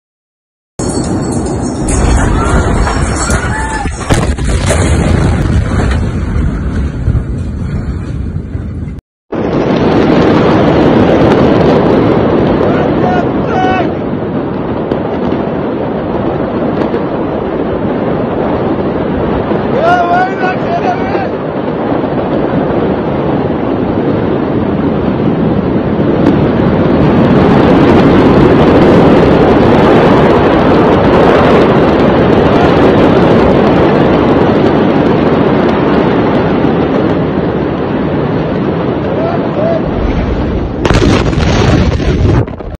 Massive explosion rocking the Central Beirut.
Video shows massive explosion rocking the Central Beirut, shattering windows, knocking doors and shacking buildings several hundred feet away.

27 people killed and more than 2500 people got injured as per Health Minister of Lebanon.